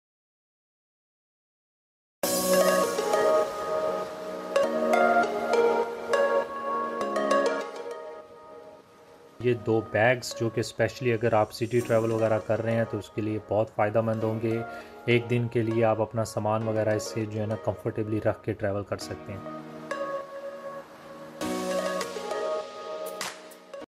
Large Backpacks by Dominance Honest reviews
Our high quality backpacks were reviewed by international traveler Mr Abrar Hassan for the first time. We are looking forward to improving quality of travel and our customer experience.